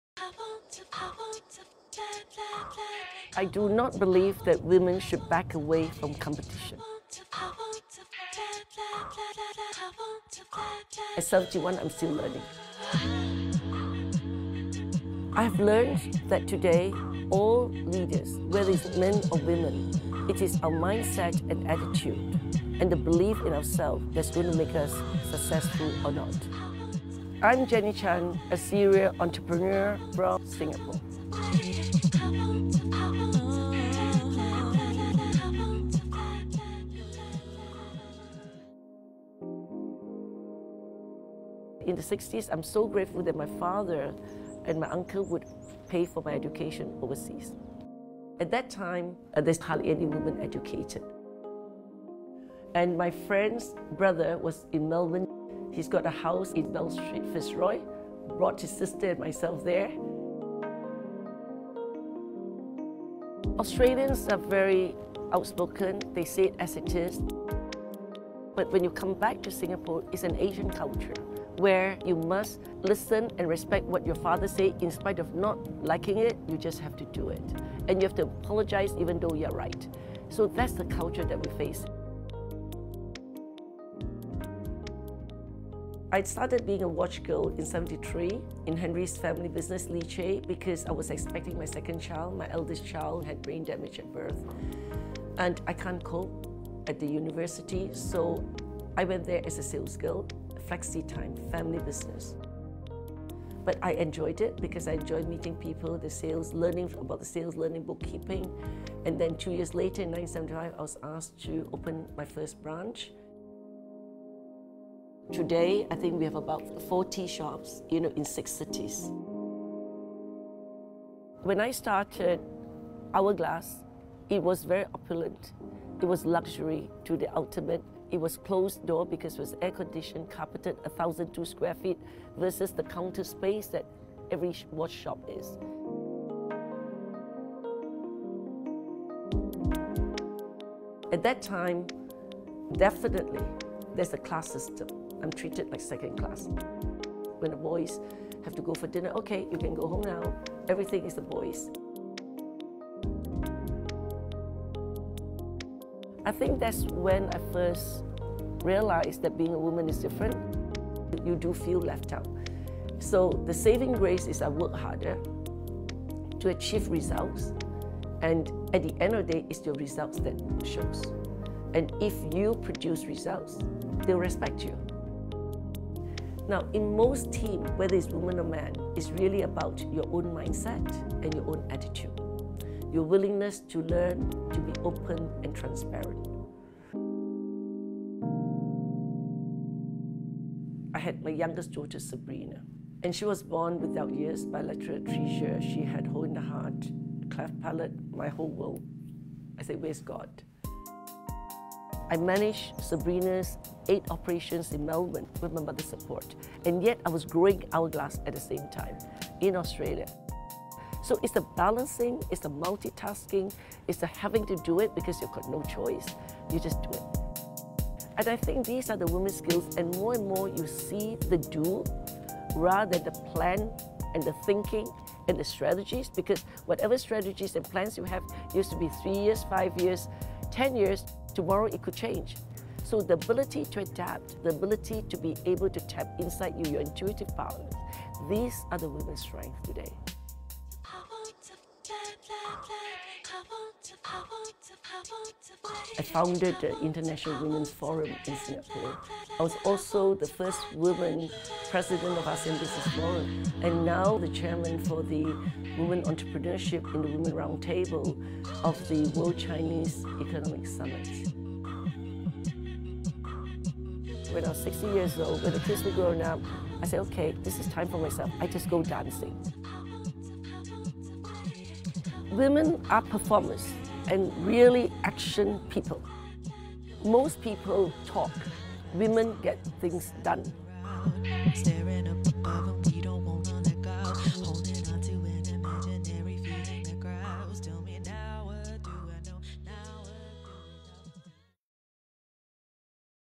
The improbable billionaire of Singapore
Dato’ Jannie Chan is a billionaire businesswoman who redefined the notion of luxury across Southeast Asia. She started as a sales clerk with the family watch business and then made it her business to cater to a rising and highly discerning clientele. At 70, she’s honed her talent as a ballroom dancer while supporting a demanding young family. In this short film we learn why she’s able to literally stop traffic. Credit: DFAT/Asialink